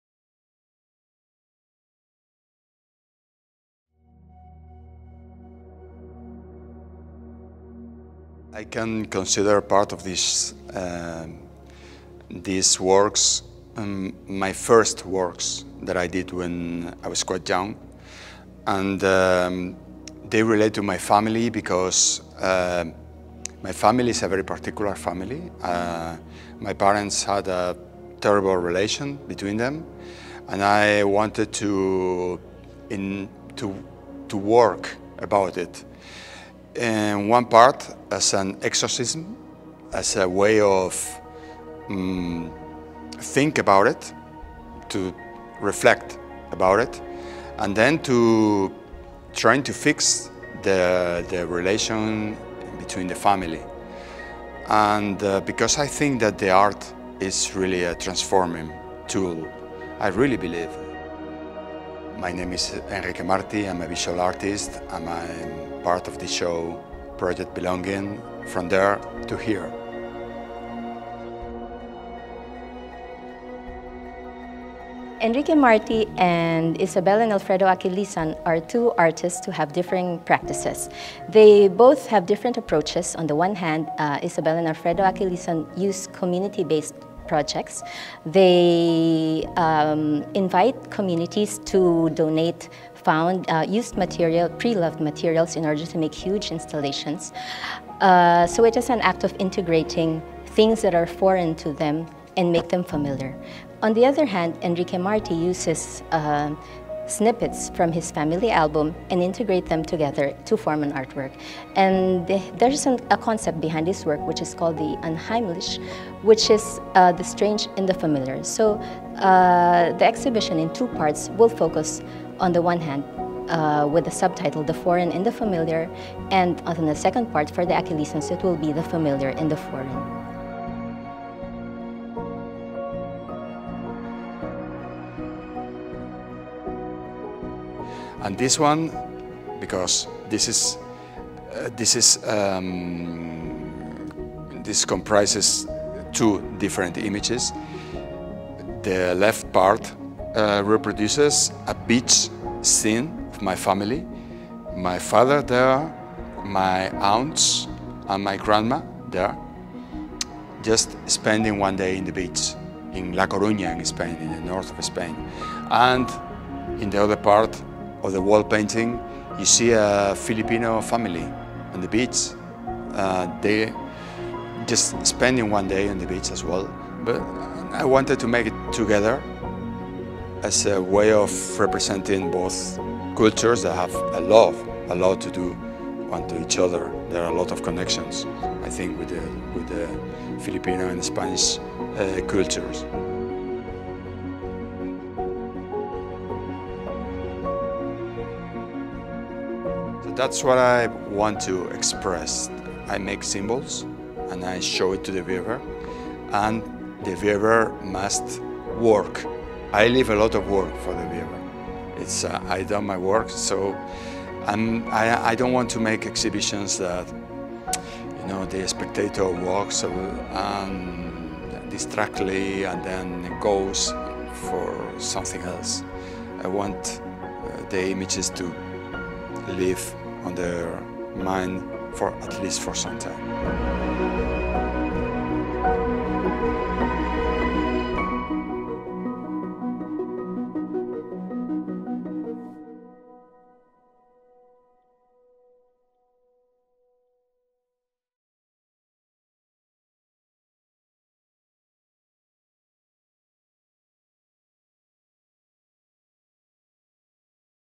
Project Belonging: From There to Here. The Foreign in the Familiar | Exhibition Highlights
“Project Belonging” is based on a dual approach of self-representation, considering the self as a product of society, emerging from social and symbolic interaction. The idea of belonging is explored by delving into the contrast of the homelike and unhomely and how both intermingle and merge.

Though Enrique Marty and Isabel & Alfredo Aquilizan define this differently in their respective practices, the figure of the family or the community take a central role in both.

The first part of the exhibition, “The Foreign in the Familiar” focuses on Enrique Marty’s work. Since the start of his career, Marty’s main body of work has been nourished by a long pictorial tradition influenced by the Baroque, and is centered on the idea of family in which the everyday and the familiar becomes a genre with its own personality.

“Project Belonging: From There to Here. The Foreign in the Familiar” is on-view from 10 August – 26 October 2024 at the second floor, Wilson L Sy Prints & Drawings Gallery, Ateneo Art Gallery, Areté, Ateneo de Manila University. This exhibition is supported in part by a grant from Acción Cultural Española (AC/E), a state agency.

📸 Video by Clefvan Pornela

Follow us for more updates on our programs!